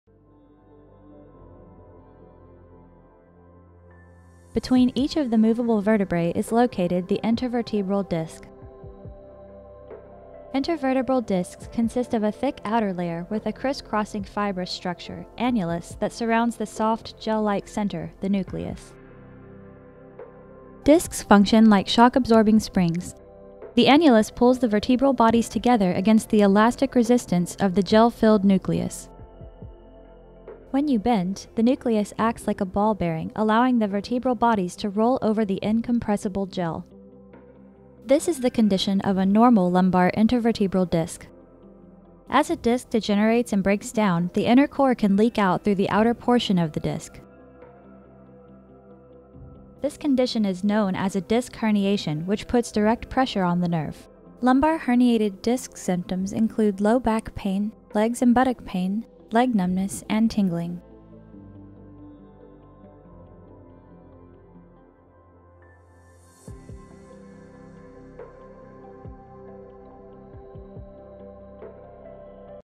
What is Lumbar Disc Herniation? | Herniated Disc
Learn more about Lumbar Intervertebral Disc Herniation and its treatment options

A herniated disc happens when the inner disc material, called the nucleus, seeps through the tough outer wall of the disc. It becomes symptomatic when it comes into contact with surrounding nerves causing pain and other herniated disc symptoms to be felt locally and along the nerves path.
In a healthy disc you can notice that the outer wall is thick and tough. As disc degeneration starts to set in, this wall will start to thin and dry out allowing for the possibility of cracks or tears. Once a tear or a crack occurs the disc can herniate as the inner materials come through and places pressure on surrounding structures.